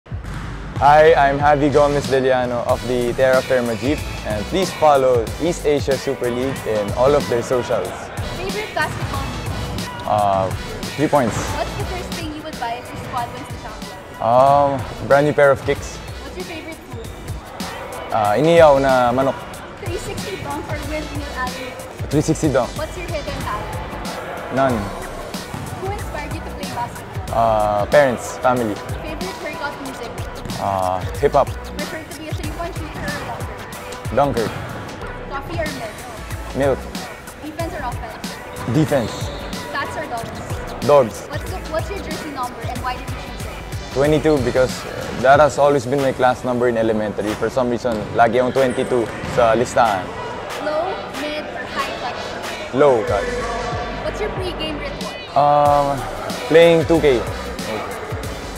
EASL Fastbreak with Terrafirma Dyip's Javi Gomez de Liano
Say hi to @javigdl22 as he shares more about himself through a quick EASL Fastbreak 🎙EASL